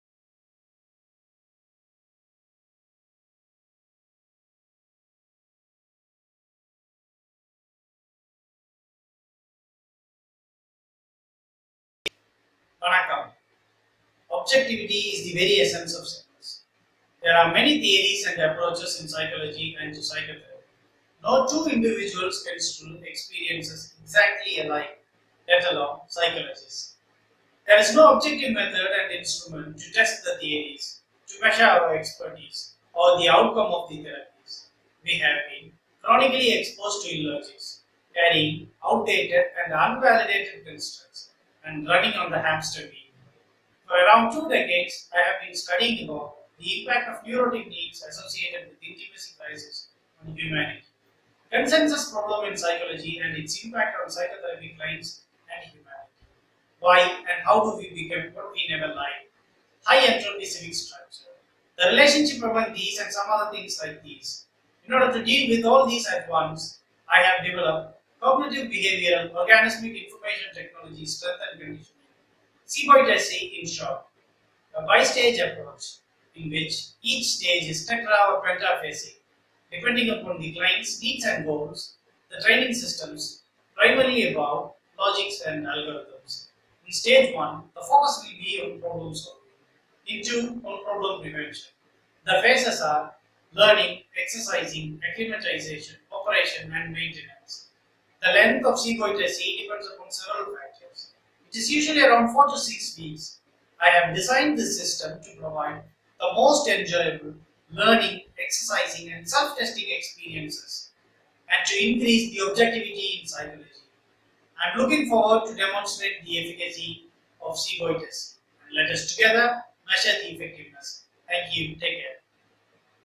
CBOITSC Introduction
Cognitive-Behavioural Organismic Information Technology Strength and Conditioning (CBOITSC) is a bistage approach and is primarily about logics and algorithms.